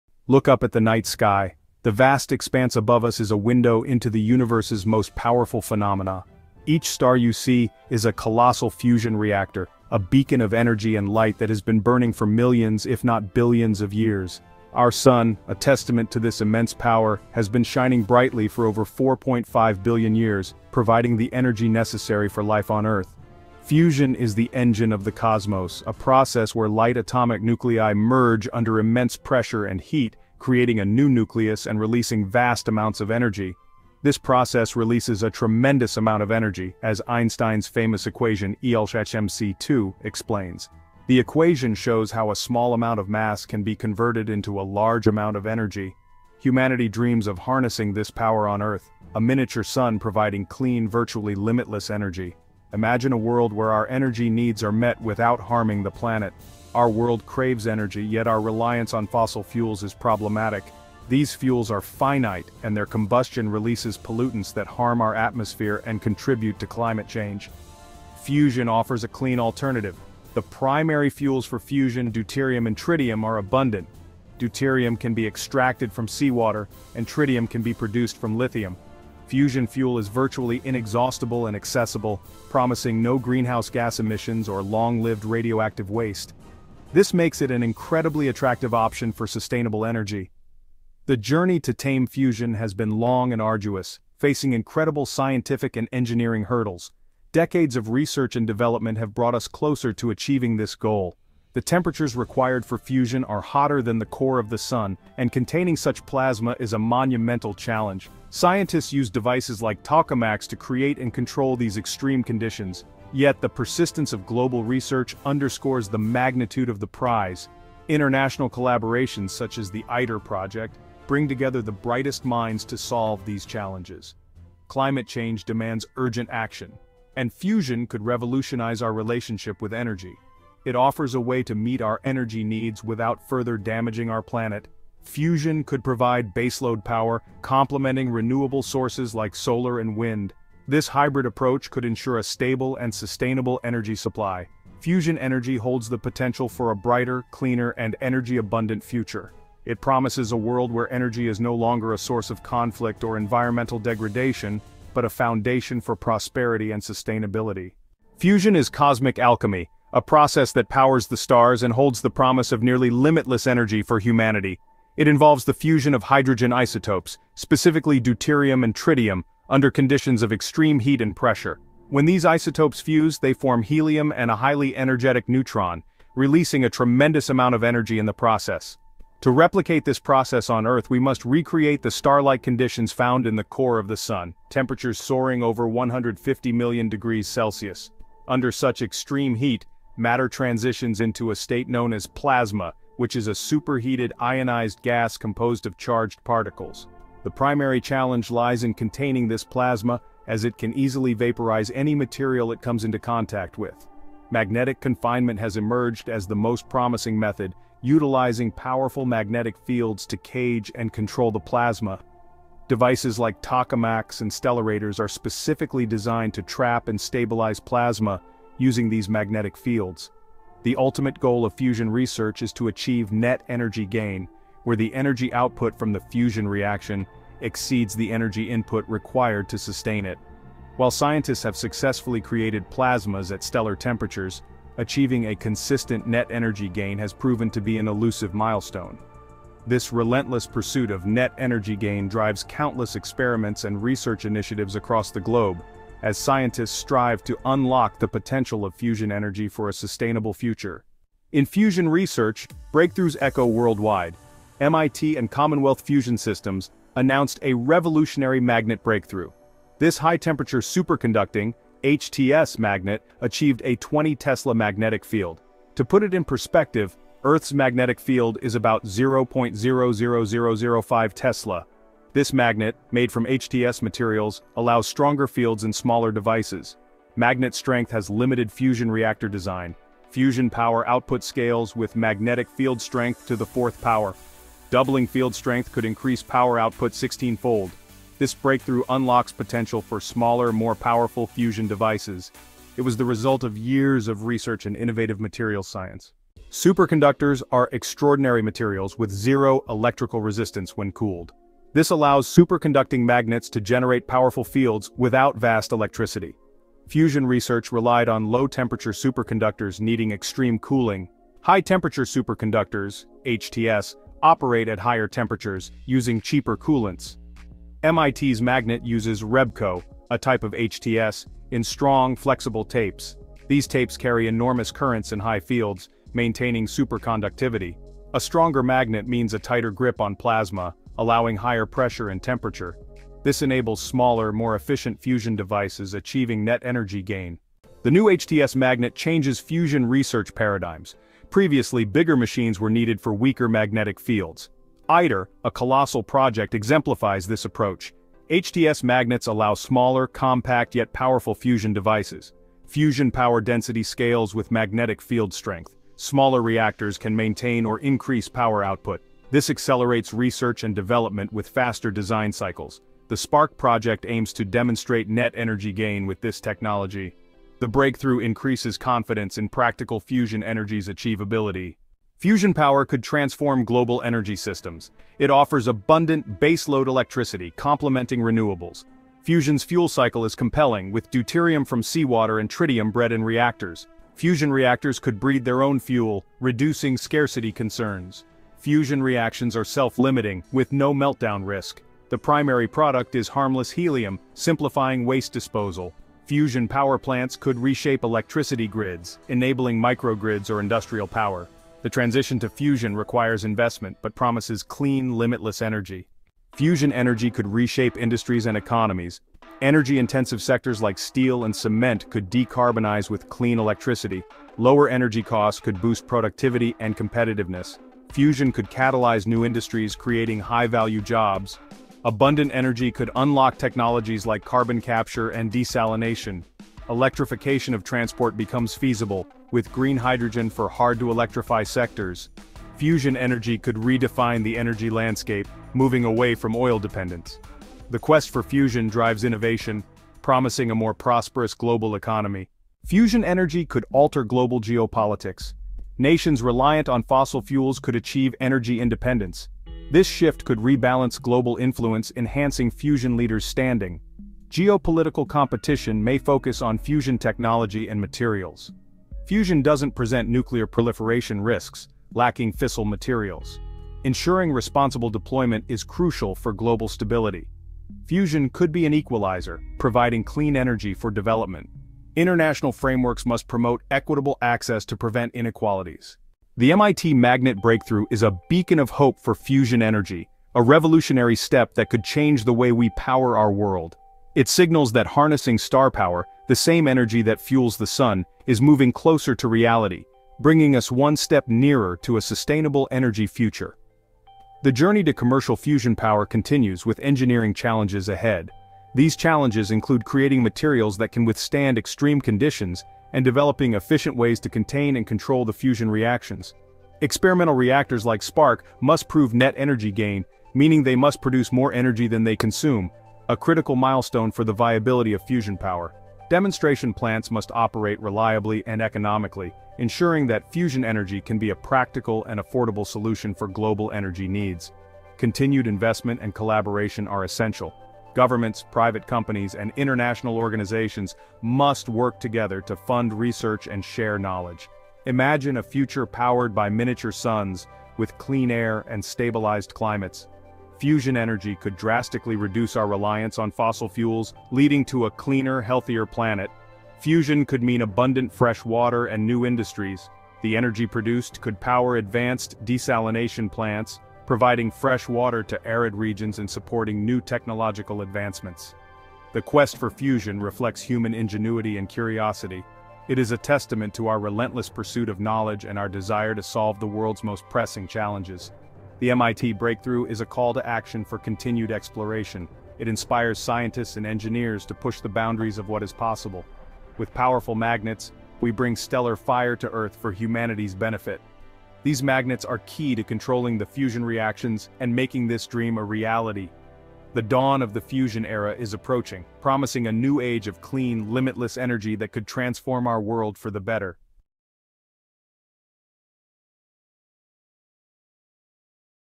Nuclear Fusion Unlocked by MIT – A Tipping Point for the Oil Industry?
MIT has achieved a groundbreaking milestone in nuclear fusion, potentially unlocking a near-limitless, clean energy source. This breakthrough could drastically reduce the world’s reliance on oil and other fossil fuels. The implications stretch beyond science, threatening to disrupt the global energy market. Fusion energy, once a distant dream, is now closer than ever to reality. The oil industry may be facing its greatest existential challenge yet.